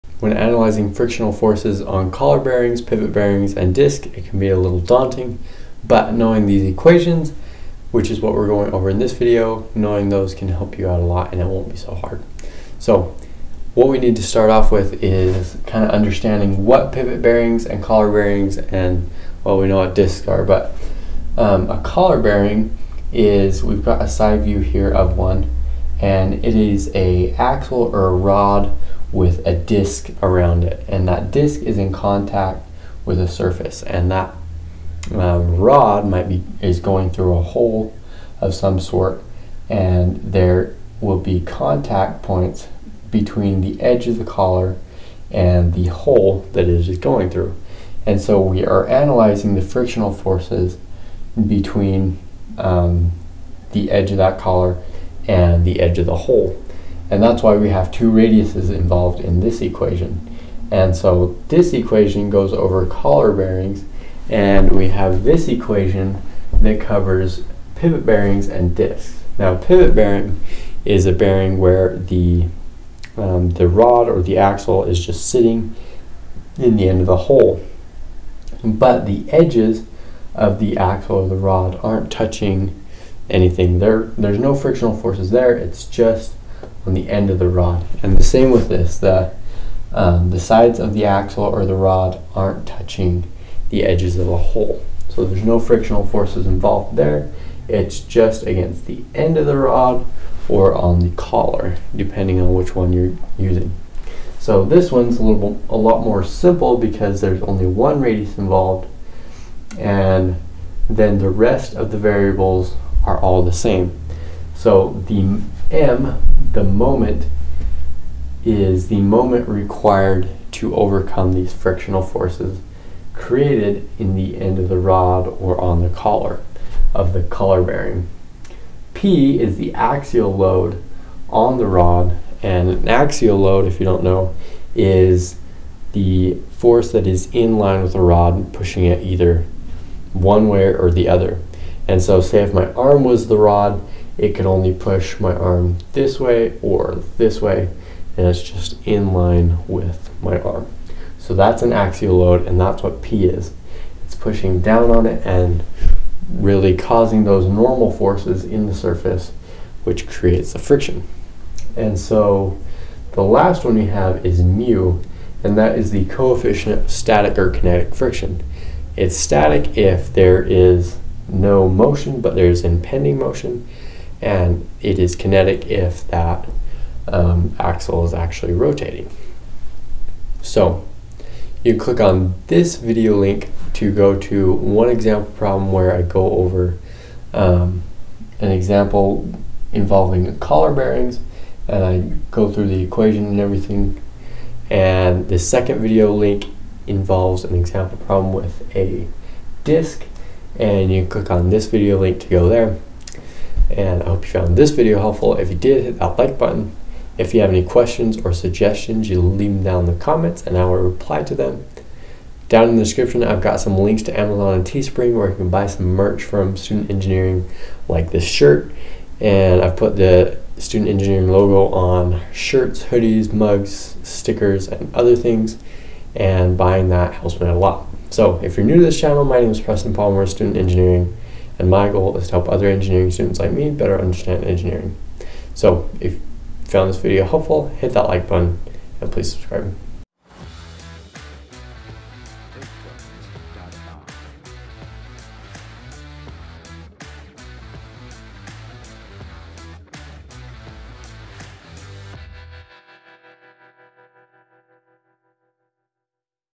Frictional Forces on Collar Bearings, Pivot Bearings and Disks // Equations Given and Explained
In this video I describe and show what collar bearings, pivot bearings and disks are and how to analyze the frictional forces involved with each of them.